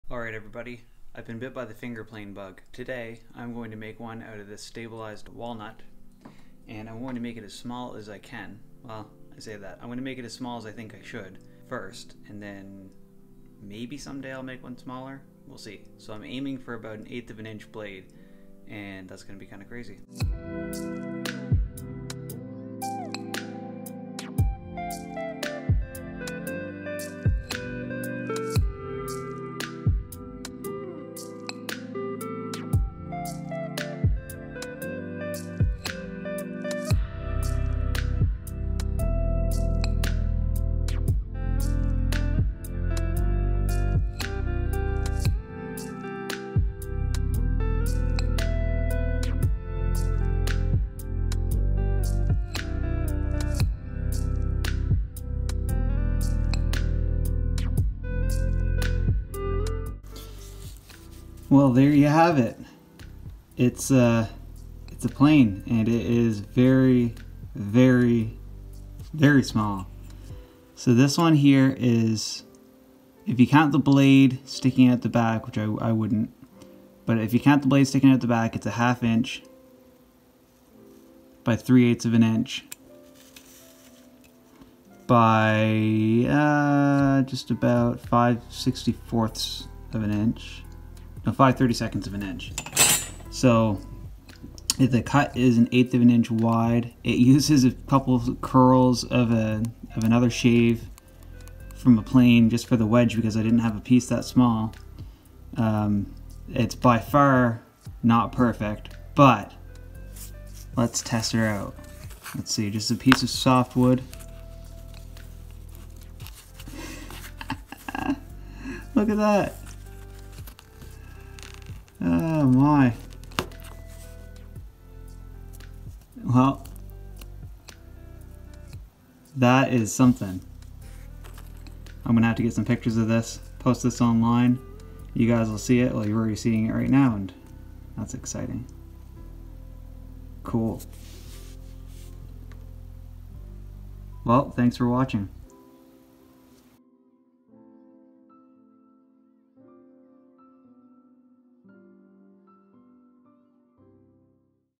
Worlds Smallest Wood Plane
In this video I make the worlds smallest (to my knowledge) wood plane. its made with my super simple wood plane building tutorial

Wooden planes are used to make lots of things, they are a very versatile tool to have, this one however, is more or less useless. But not all things have to have a purpose, they can be useless too.